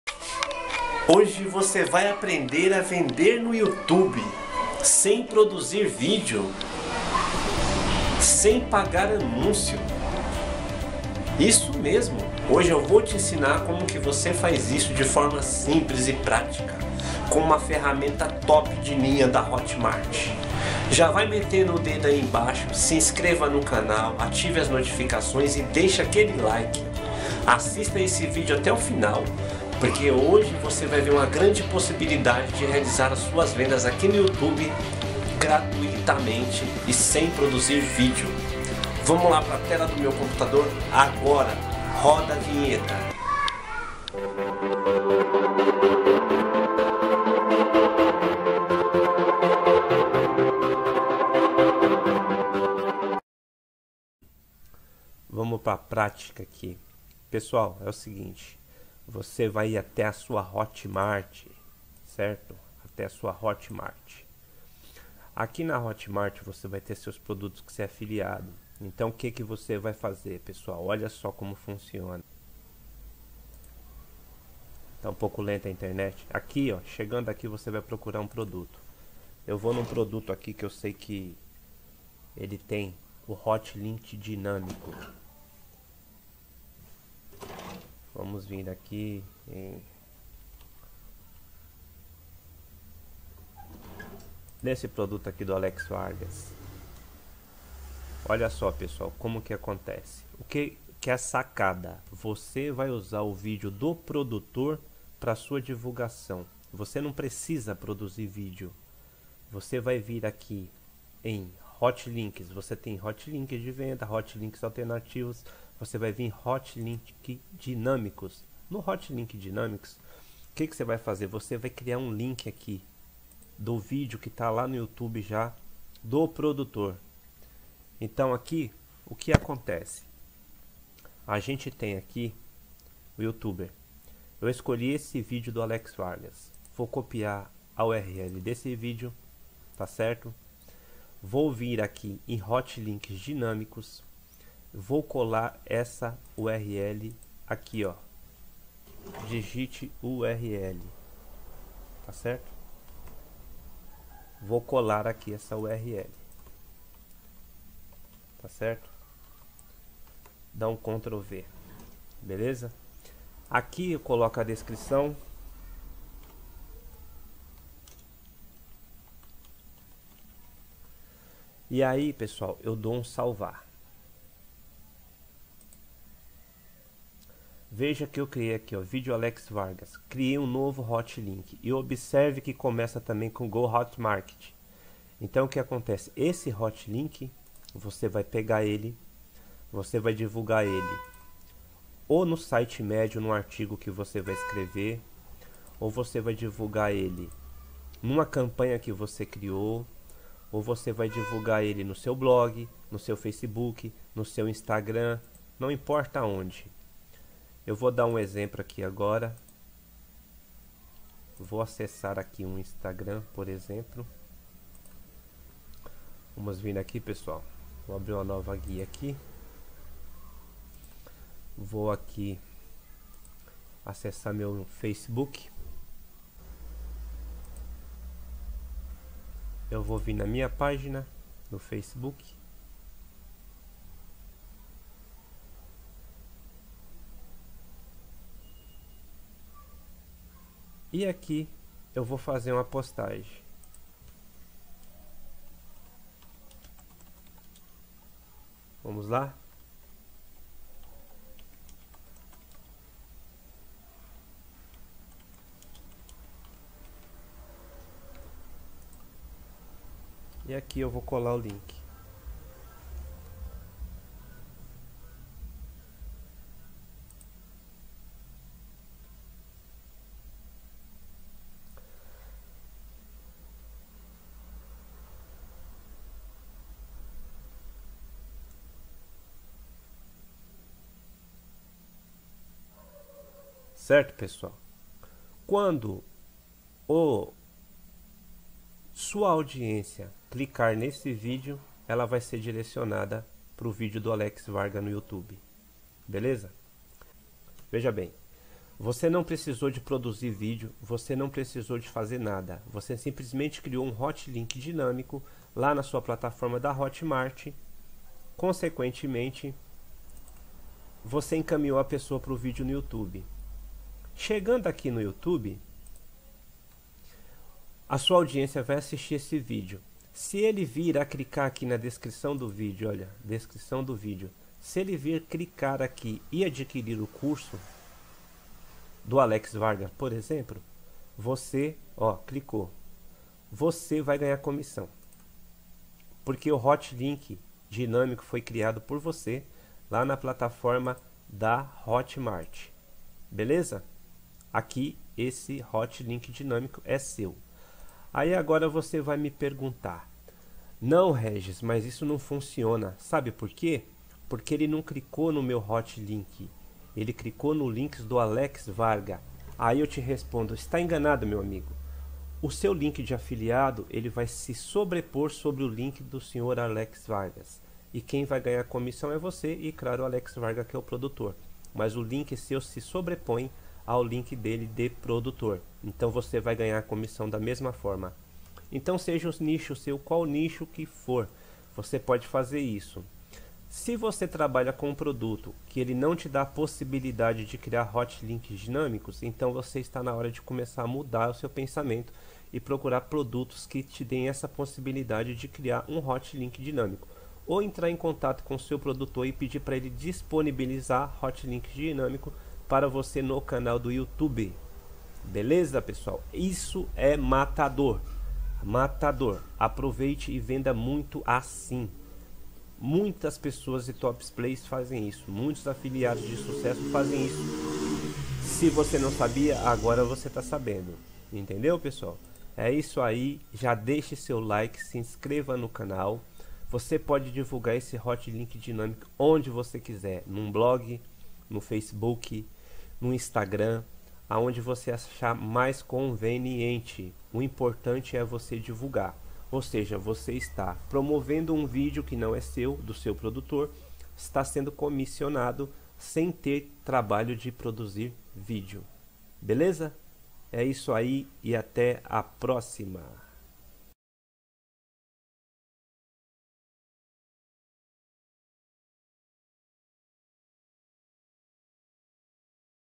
Vender como afiliado no youtube sem produzir vídeo
Você vai aprender  usar uma ferramenta disponível na Hotmart
que vai fazer você vender no youtube sem produzir vídeo, assista esse vídeo até o final e veja como o caminho do marketing digital pode ser fácil se você
estudar e aprender a usar  todas ferramentas disponíveis.
Então aprenda de uma vez por todas a vender como afiliado no youtube sem produzir vídeo

➡ Ebook Grátis com o Passo a Passo de Como aumentar seu tráfego